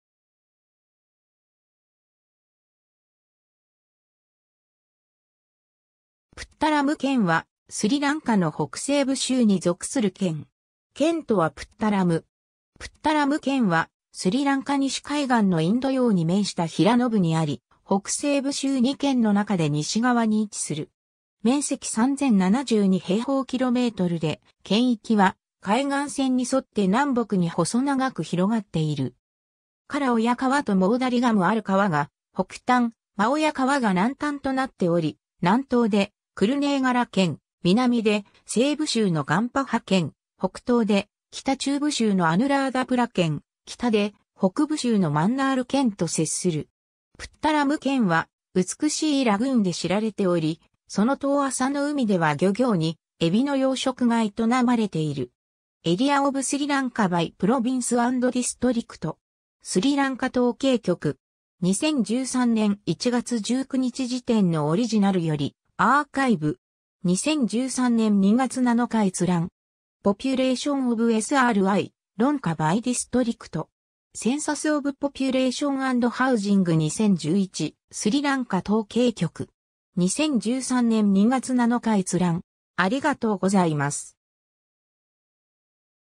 プッタラム県
北西部州_(スリランカ)
プッタラム県（プッタラムけん、シンハラ語: පුත්තලම දිස්ත්‍රික්කය、タミル語: புத்தளம் மாவட்டம்、英語: Puttalam District）は、スリランカの北西部州に属する県。
県都はプッタラム。
 プッタラム県はスリランカ西海岸のインド洋に面した平野部にあり、北西部州2県の中で西側に位置する。
面積3,072 km2で、県域は海岸線に沿って南北に細長く広がっている。
Kala Oya川とModaragam Aru川が北端、Ma Oya川が南端となっており、南東でクルネーガラ県、南で西部州のガンパハ県、北東で北中部州のアヌラーダプラ県、北で北部州のマンナール県と接する。
 プッタラム県は美しいラグーンで知られており、その遠浅の海では漁業にエビの養殖が営まれている。
 ^ “Area of Sri Lanka by province and district (PDF)” (英語,シンハラ語,タミル語). スリランカ統計局 (2010年). 2013年1月19日時点のオリジナルよりアーカイブ。
2013年2月7日閲覧。
 ^ “POPULATION OF SRI LANKA BY DISTRICT (PDF)” (英語,シンハラ語,タミル語). Census of Population and Housing 2011. スリランカ統計局 (2012年4月20日). 2013年2月7日閲覧。